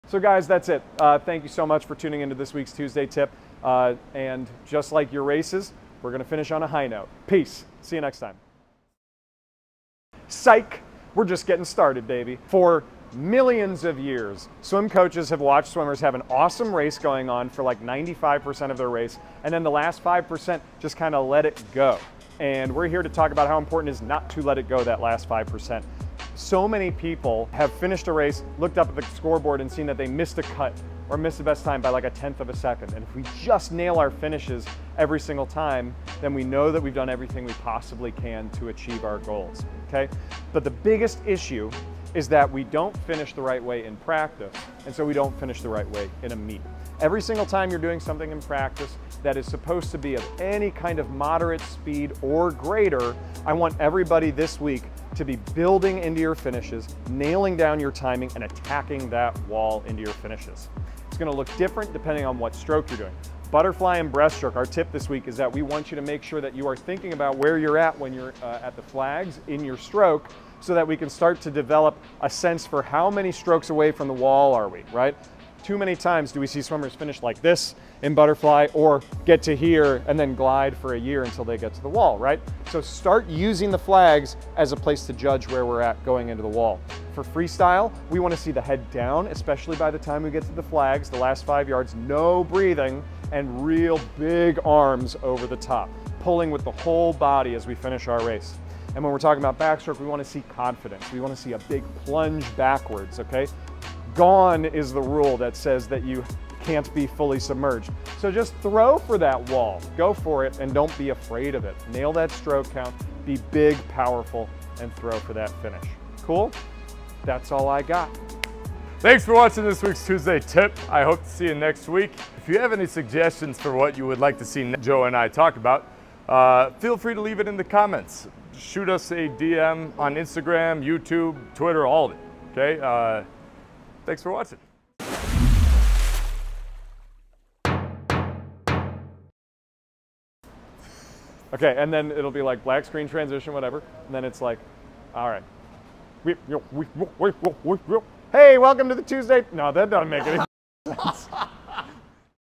TuesdayTip 042: Practicing your Finishes!
One of our biggest blind spots when it comes to racing is getting our hand(s) on the wall first! This week, Coach Joe talks about what we need to do in our workouts to make sure we're prepared for our weekend throw-downs. Good luck this week and in your upcoming meets!

0:00 Intro
0:15 Practicing like you race
1:03 Breast & Fly
1:27 Freestyle
1:38 Backstroke
1:58 Outro